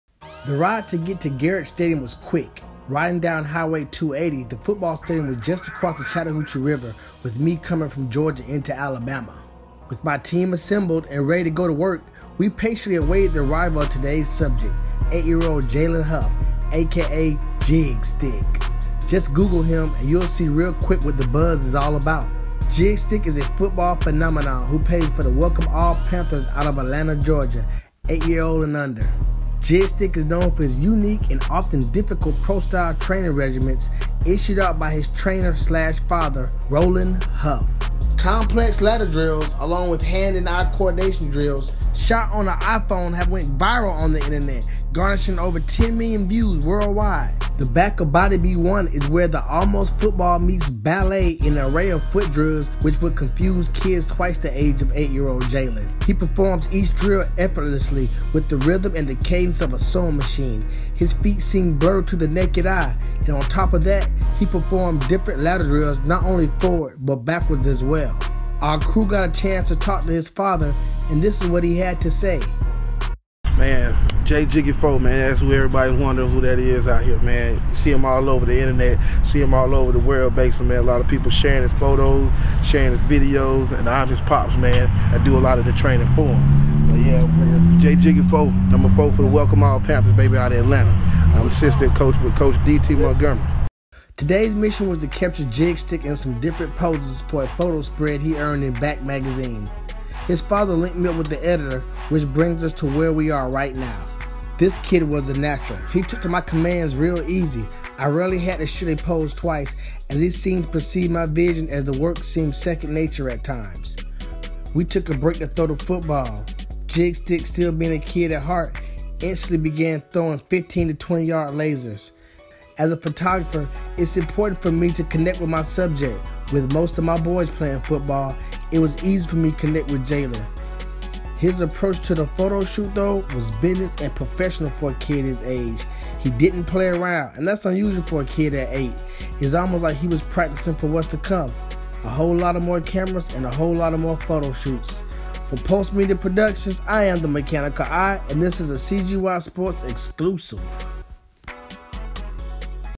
Jaylen Huff aka Jigstick (Behind The Scenes) Photoshoot with CGYSports
Behind the scenes look of a photoshoot with Jaylen Huff aka Jigstick